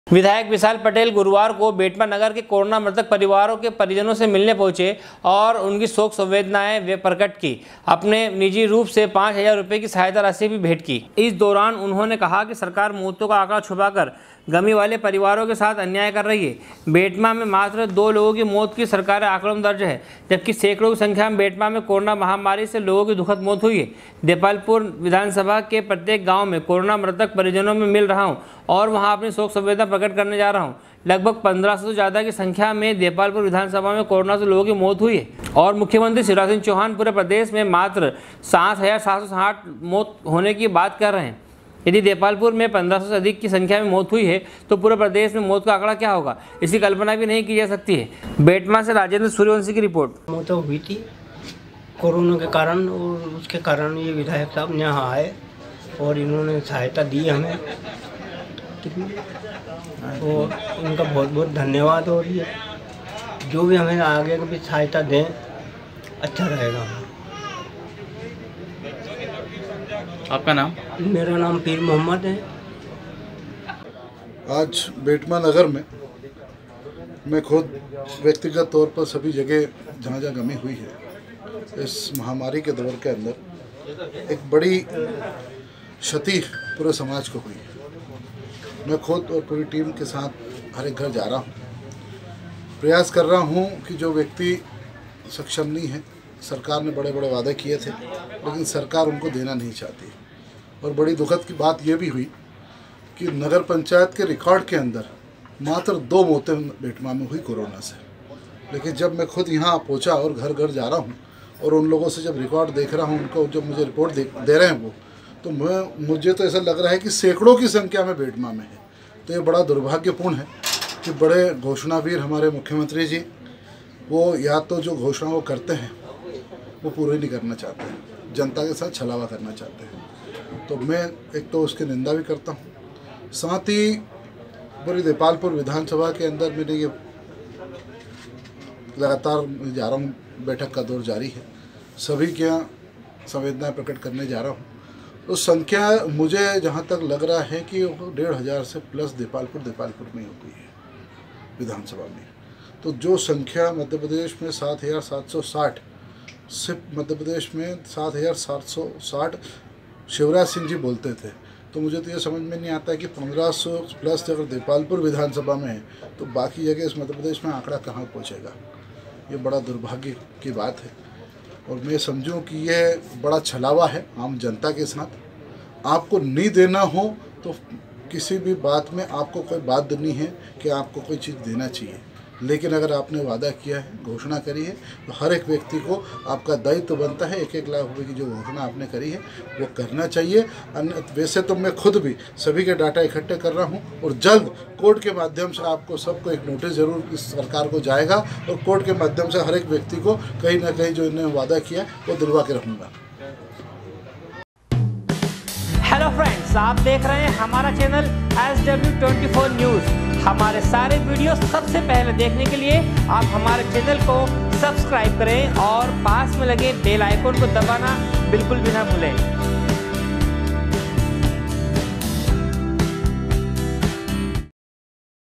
betma- गुरुवार को बेटमा नगर में कोरोना मृतक व्यक्ति के परिवार जनों से मिलने पहुँचे
विधायक विशाल पटेल गुरुवार को बेटमा नगर में कोरोना मृतक व्यक्ति के परिवार जनों से मिलने पहुँचे ओर अपनी शोक संवेदनाएं प्रकट की साथ ही अपने निजीरूप से 5 हजार रुपए की सहायता राशी भी भेंट की। बेटमा से राजेन्द्र सुर्यवंषी की रिपोर्ट